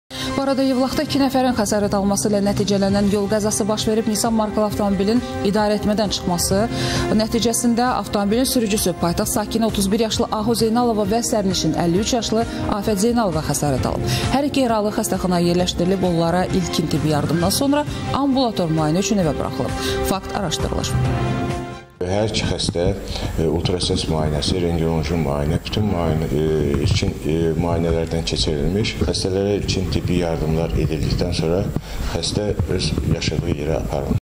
Minik maşını aşdı, 2 nəfər xəsarət aldı
Yevlaxda 2 nəfərin xəsarət alması ilə nəticələnən yol qəzası baş verib. "Nissan" markalı avtomobilin idarəetmədən çıxaraq aşması nəticəsində avtomobilin sürücüsü, paytaxt sakini, 31 yaşlı Ahu Zeynalova və sərnişin, 53 yaşlı Afət Zeynalova xəsarət alıblar. Hər iki yaralı xəstəxanaya yerləşdirilib. Onlara ilkin tibbi yardımdan sonra ambulator müayinə üçün evə buraxılıb. Faktla bağlı araşdırma aparılır